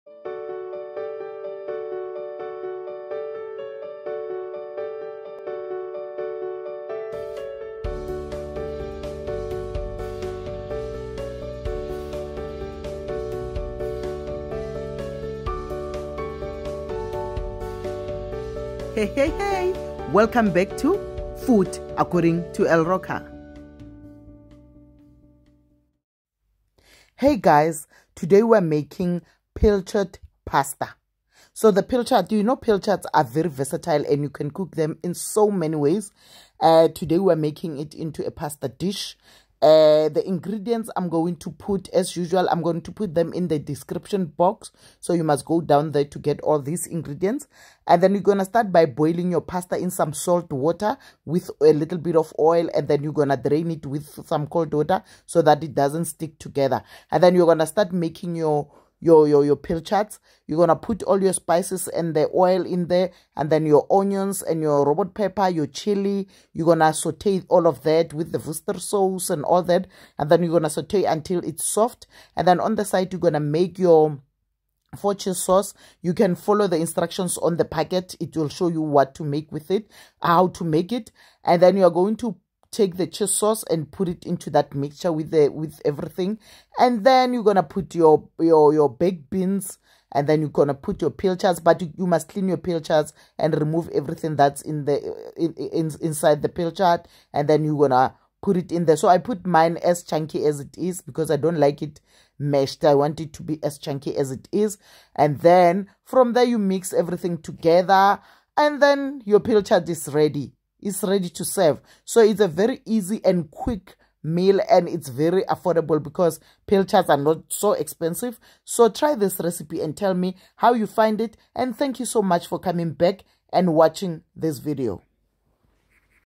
PILCHARD PASTA RECIPE
Ingredients

Pasta
Pilchards
Royco 4 cheese sauce
Robot peppers
Jalapeno chilli
Red onion
Baked beans
Course black pepper
Parsley
Worstersauce
Ginger
Garlic
Oil